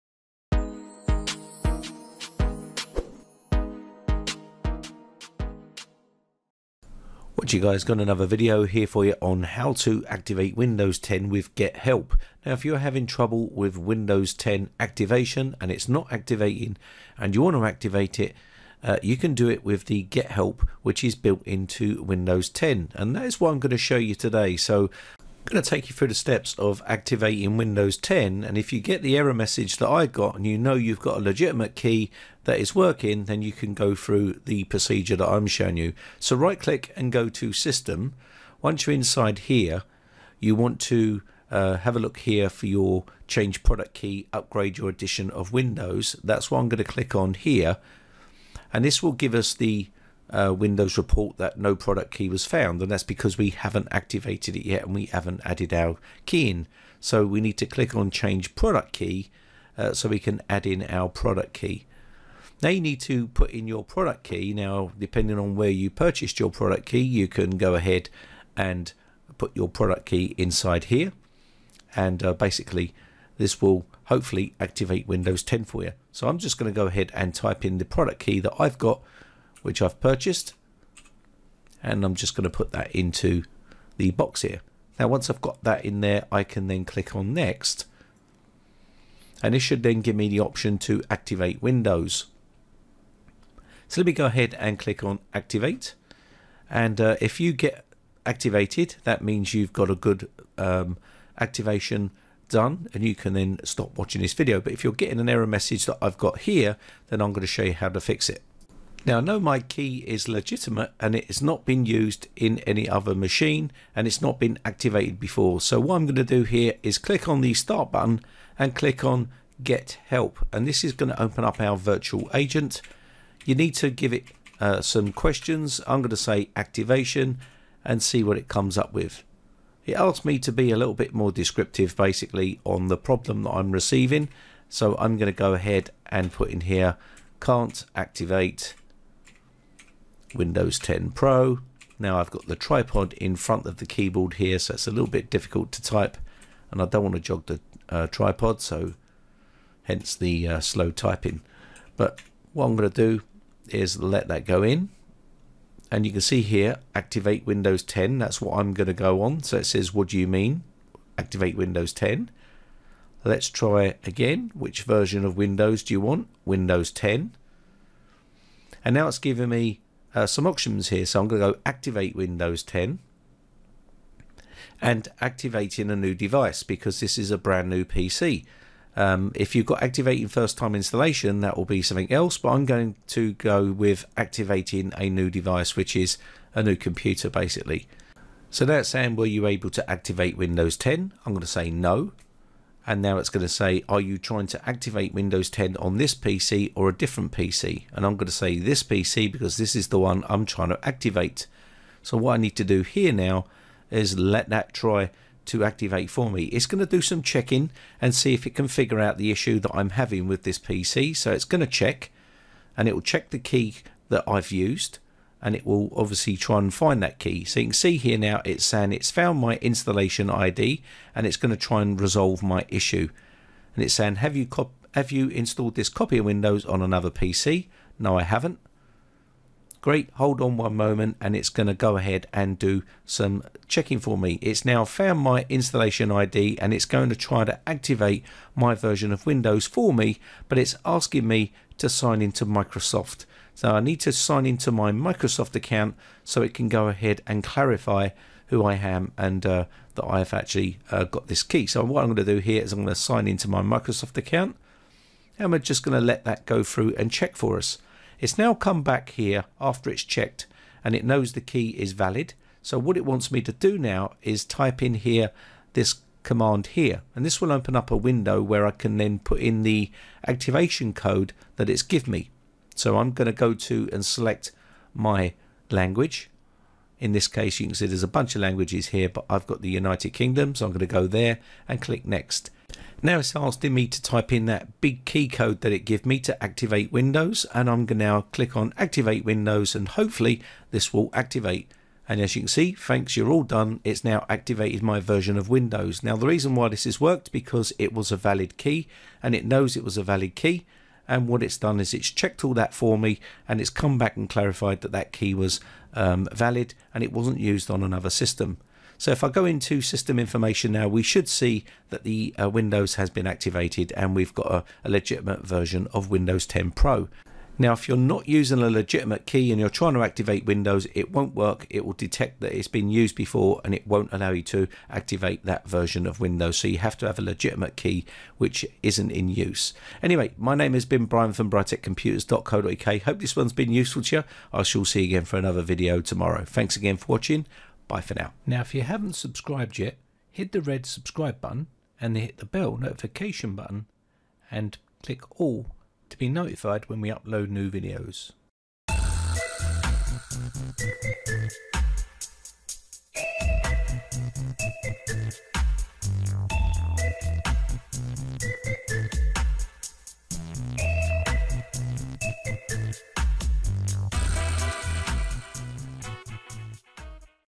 How to Fix Windows 10 Product Key Activation Not Working
In this video I will show you how to fix Windows 10 activation error. As long as you have a legitimate Windows 10 product key that has not been used on another computer, then I will show you how to get your windows 10 product key validated and activated. This is a genuine way to activate windows 10. You can activate Windows 10 on your computer, via the telephone or you can use the get help feature built into Window, this will use the virtual agent to try and resolve your windows 10 activation problem.

Important: Make sure you are using a legit Windows 10 Key and that the key has never been used on another computer.